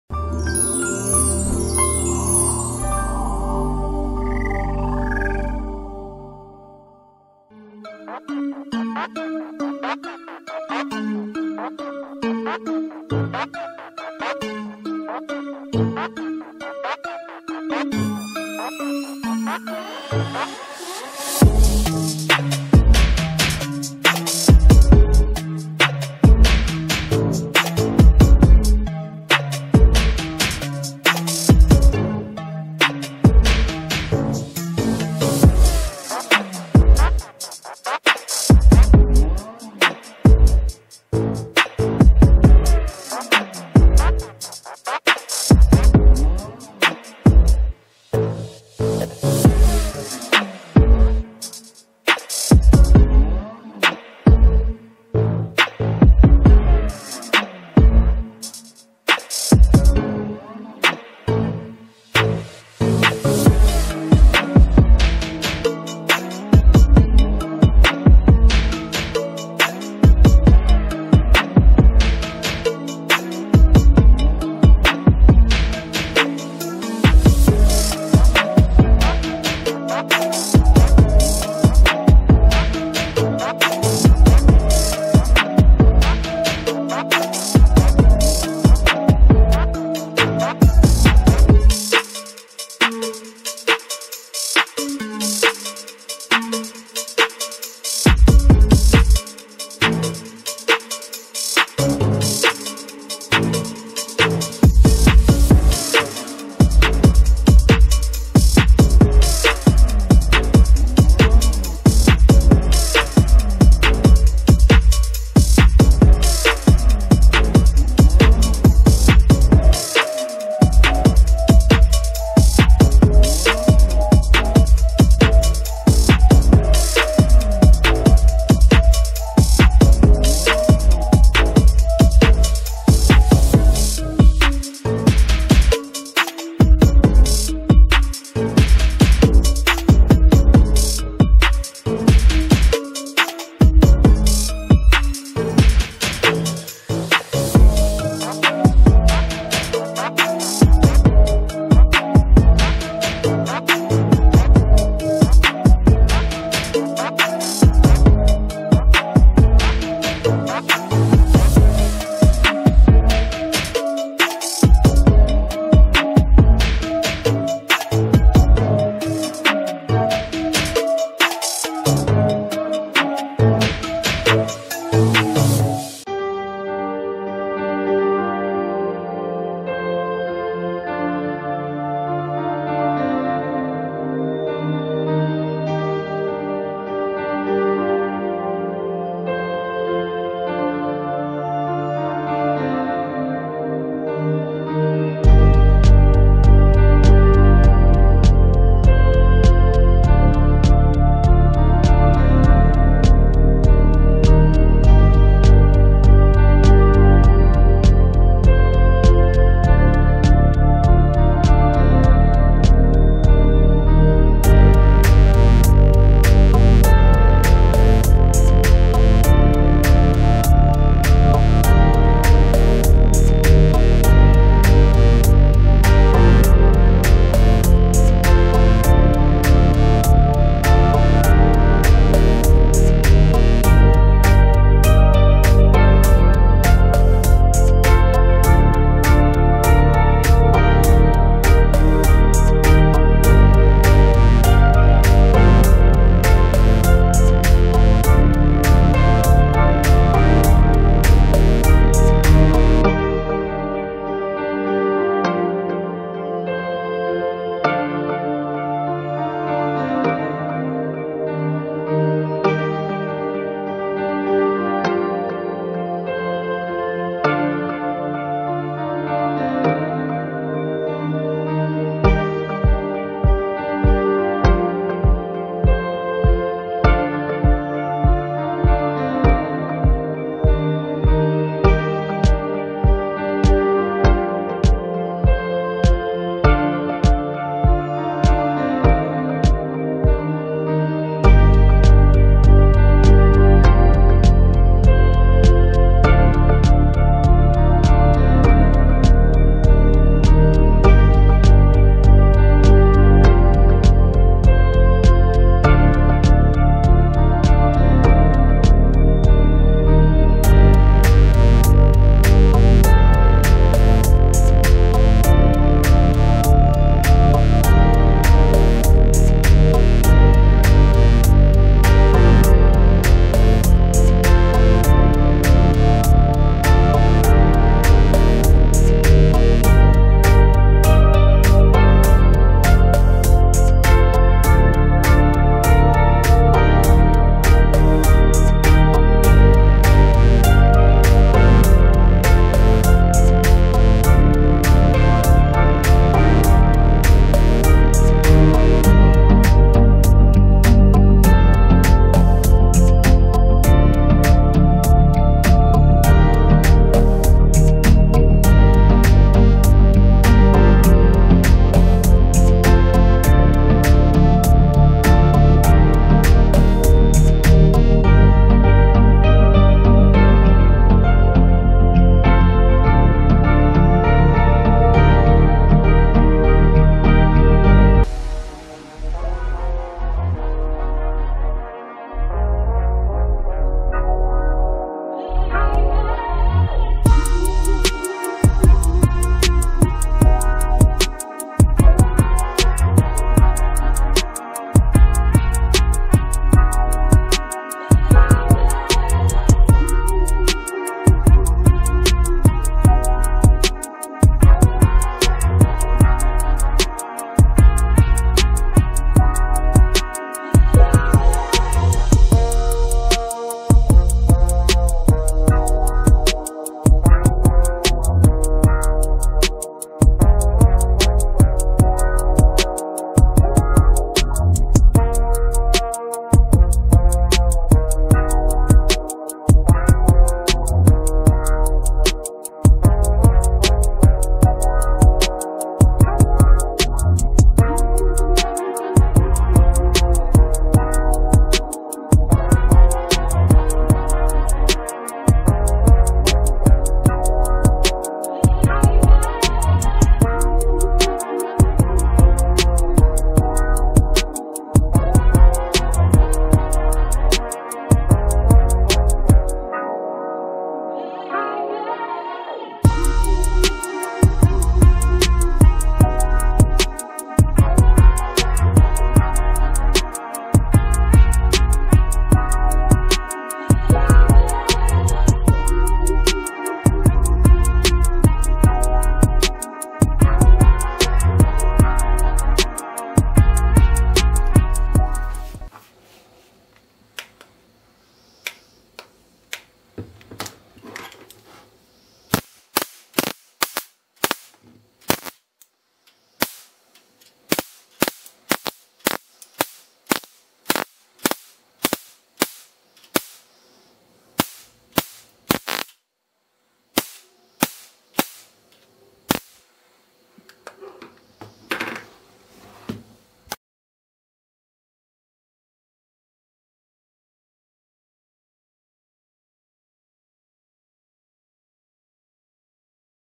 How To Replace Battery on Mosquito Killer Bat With Lithium Battery For Long Backup
Hello friends, this is an idea to repair your mosquito killer bat.  Actually inbuilt d battery does not give proper out and this battery goes faulty quickly but this lithium ion battery will give you morr time to operate this bat.